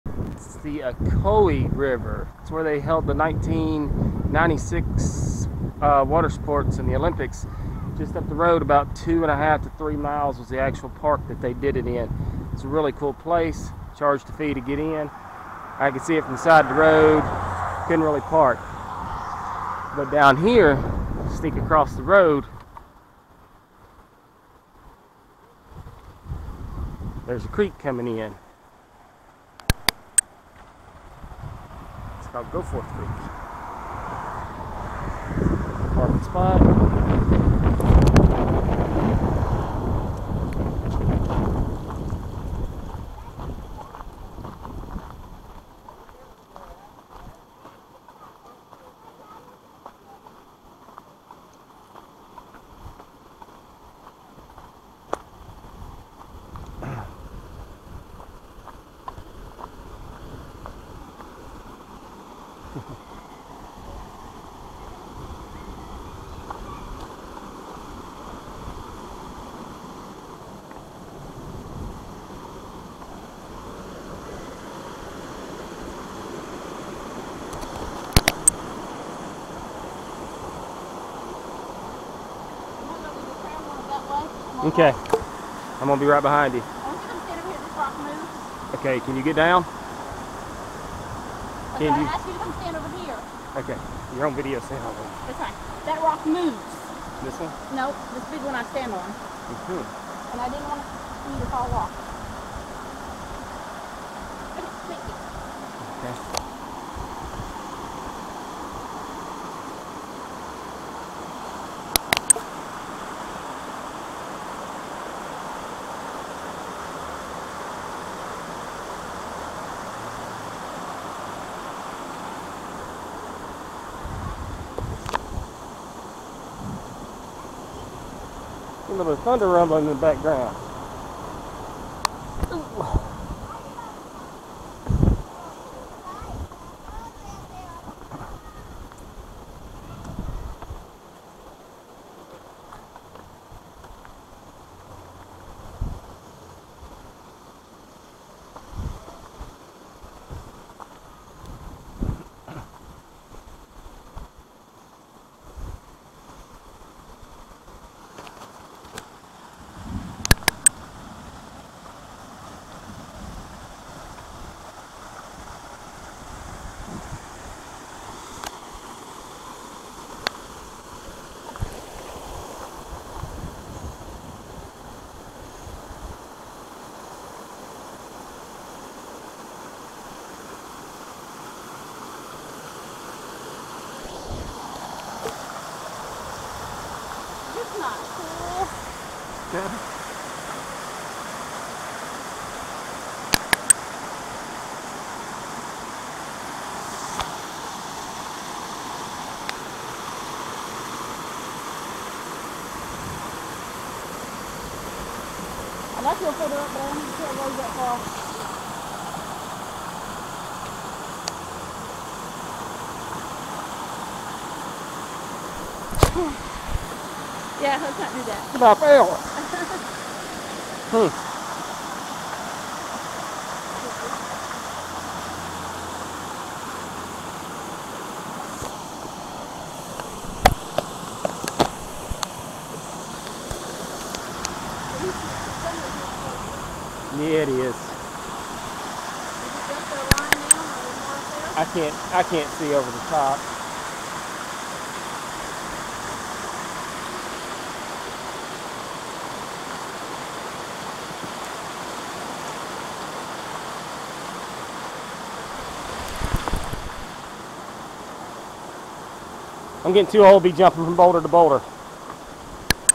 Goforth Creek
Ocoee River ( sight of 1996  Olympic White Water Events) and a side trip of a stream flowing into it.